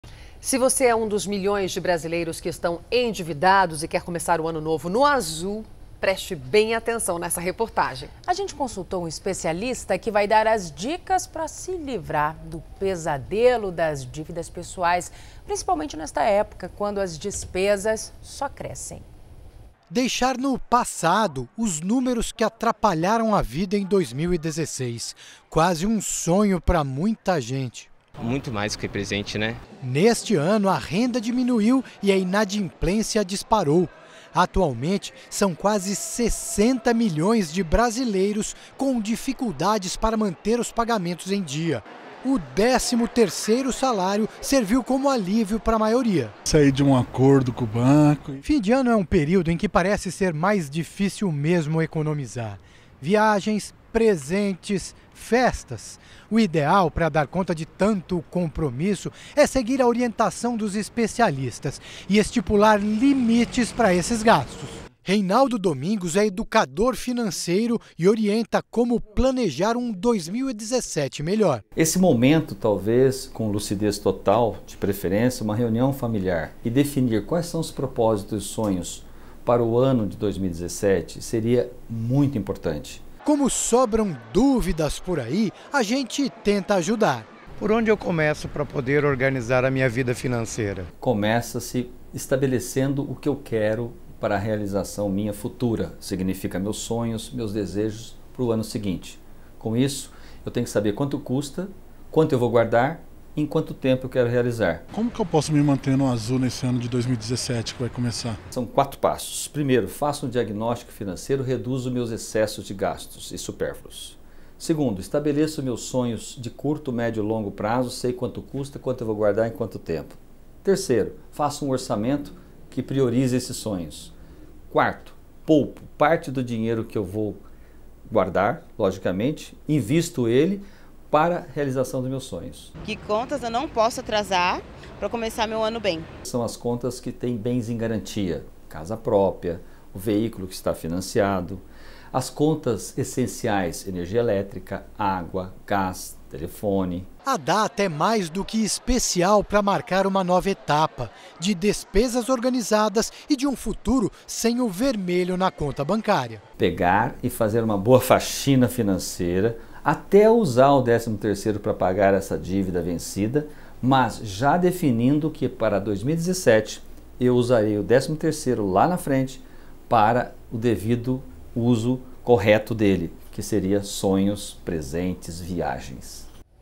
Confira dicas para começar o ano novo livre das dívidas pessoais
Deixar no passado os números que atrapalharam a vida em 2016 é o sonho de muita gente, principalmente se você é um dos milhões de brasileiros endividados que desejam começar o ano novo no azul. Um especialista deu dicas para se livrar do pesadelo das dívidas pessoais.